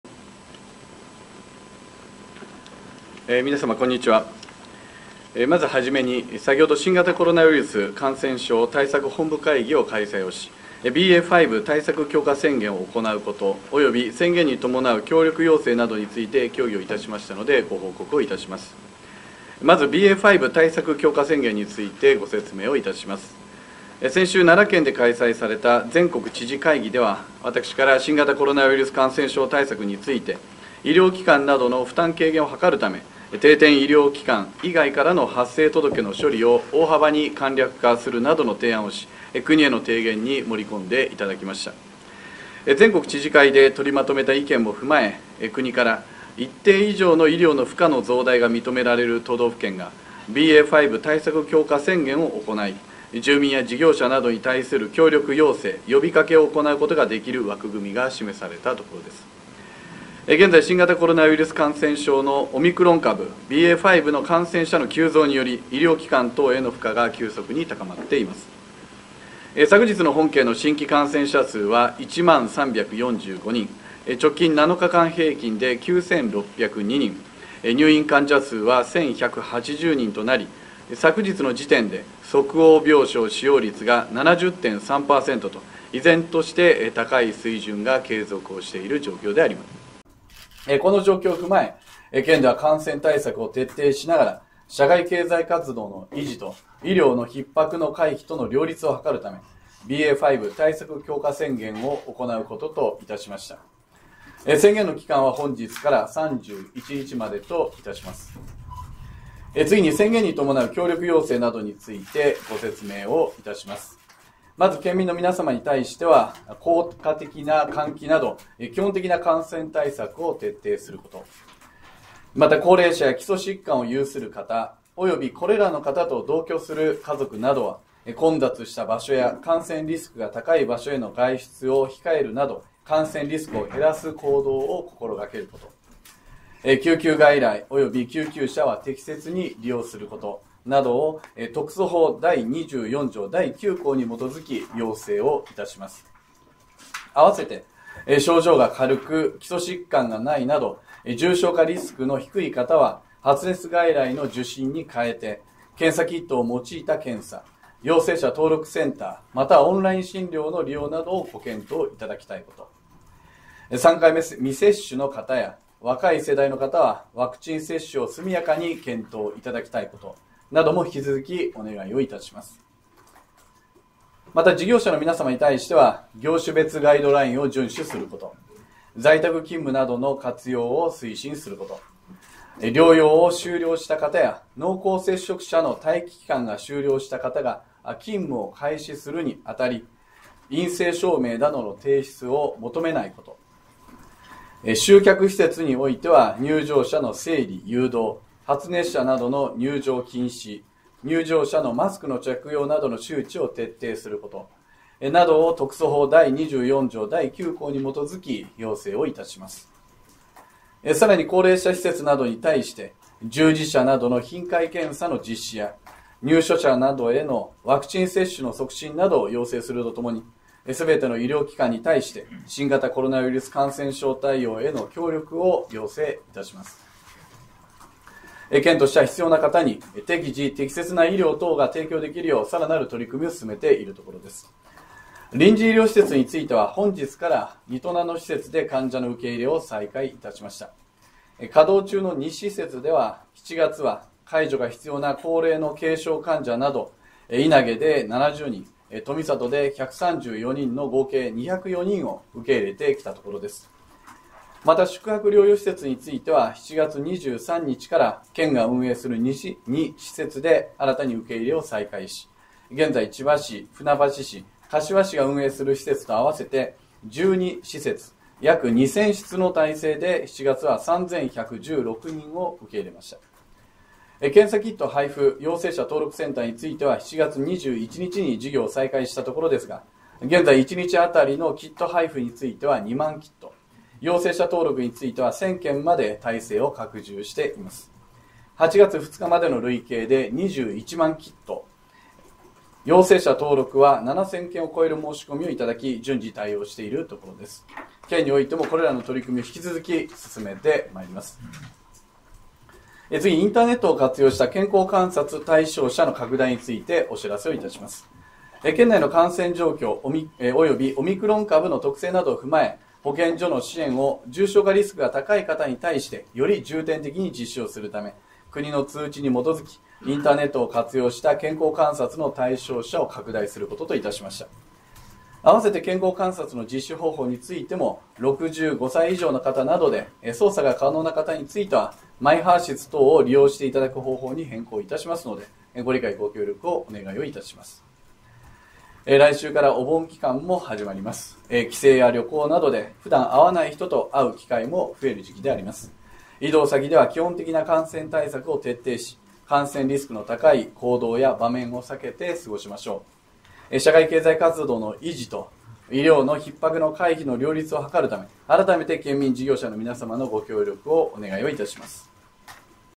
8月4日｜BA.5対策強化宣言について知事メッセージ（定例会見）
【新型コロナウイルス感染症への対応について】
はじめに、先ほど新型コロナウイルス感染症対策本部会議を開催し、「BA.５対策強化宣言」を行うこと及び宣言に伴う協力要請等について協議しましたので、ご報告します。

（BA.5対策強化宣言について）
まず、「BA.５対策強化宣言」について御説明いたします。
先週、奈良県で開催された「全国知事会議」では、私からは、新型コロナウイルス感染症対策について、医療機関等の負担軽減を図るため、定点医療機関以外からの発生届の処理を大幅に簡略化するなどの提案をし、国への提言に盛り込んでいただきました。
全国知事会で取りまとめた意見も踏まえ、国から、一定以上の医療の負荷の増大が認められる都道府県が「BA.５対策強化宣言」を行い、住民や事業者等に対する協力要請・呼びかけを行うことができる枠組みが示されたところです。
現在、新型コロナウイルス感染症のオミクロン株（ＢＡ.５）の感染者の急増により医療機関等への負荷が急速に高まっています。
昨日の本県の新規感染者数は、１０，３４５人、直近７日間平均で９，６０２人、入院患者数は１，１８０人となり、昨日時点で即応病床使用率が７０．３％と依然として高い水準が継続している状況です。
この状況を踏まえ、県では、感染対策を徹底しながら社会経済活動の維持と医療のひっ迫の回避との両立を図るため、「BA.５対策強化宣言」を行うこととしました。
宣言の期間は、本日から３１日までとしました。

（BA.5対策強化宣言に伴う協力要請等について）
次に、宣言に伴う協力要請等について、御説明いたします。
まず、県民の皆さまに対しては、
　・　効果的な換気など基本的な感染対策を徹底すること
　・　高齢者や基礎疾患を有する方及びこれらの方と同居する家族等は、混雑した場所や感染リスクが高い場所への外出を控える等、感染リスクを減らす行動を心がけること
　・　救急外来及び救急車は、適切に利用すること
などを特措法第２４条第９項に基づき要請します。
併せて、
　・　症状が軽く、基礎疾患がないなど重症化リスクの低い方は、発熱外来の受診に代えて、検査キットを用いた検査、陽性者登録センター又はオンライン診療の利用などを検討いただきたいこと
　・　３回目未接種の方や若い世代の方は、ワクチン接種を速やかに検討いただきたいこと
なども引き続きお願いします。
また、事業者の皆さまに対しては、
　・　業種別ガイドラインを遵守すること
　・　在宅勤務等の活用を推進すること
　・　療養を終了した方や濃厚接触者の待機期間が終了した方が勤務を開始するに当たり、陰性証明等の提出を求めないこと
　・　集客施設においては、入場者の整理・誘導、発熱者等の入場禁止、入場者のマスクの着用等の周知を徹底すること
さらに、高齢者施設等に対して、従事者等の頻回検査の実施や入所者等へのワクチン接種の促進等を要請するとともに、全ての医療機関に対して、新型コロナウイルス感染症対応への協力を要請します。

（医療提供体制の強化等の取組について）
県としては、必要な方に適時適切な医療等が提供できるよう、さらなる取組を進めているところです。
臨時医療施設については、本日から仁戸名の施設で患者の受入れを再開しました。稼働中の２施設では、７月は介助が必要な高齢の軽症患者など、稲毛７０人・富里１３４人の２０４人を受け入れてきたところです。
また、宿泊療養施設については、７月２３日から、県が運営する２施設で受入れを再開し、現在、千葉市、船橋市、柏市が運営する施設と併せて１２施設・約２，０００室体制で７月は
３，１１６人を受け入れました。
検査キット配付・陽性者登録センターについては、７月２１日に事業を再開したところですが、現在キット配付については２万キット、陽性者登録については１，０００件まで体制を拡充しています。
８月２日までの累計で２１万キット、陽性者登録は７，０００件を超えるお申し込みをいただき、順次対応しているところです。
県においても、これらの取組を引き続き進めてまいります。

（インターネットを活用した健康観察対象者の拡大について）
次に、インターネットを活用した健康観察対象者の拡大についてお知らせします。
県内の感染状況及びオミクロン株の特性等を踏まえ、保健所の支援を重症化リスクが高い方に対してより重点的に実施するため、国の通知に基づき、インターネットを活用した健康観察の対象者を拡大することとしました。
併せて、健康観察の実施方法についても、６５歳以上の方などで、操作が可能な方については、マイハーシス等を利用していただく方法に変更しますので、ご理解、ご協力をお願いします。

（県民へのメッセージ）
来週からお盆期間も始まります。帰省や旅行などで普段会わない人と会う機会も増える時期です。移動先では、基本的な感染対策を徹底し、感染リスクの高い行動や場面を避けて過ごしましょう。
社会経済活動の維持と医療のひっ迫の回避の両立を図るため、改めて、県民・事業者の皆さまのご協力をお願いします。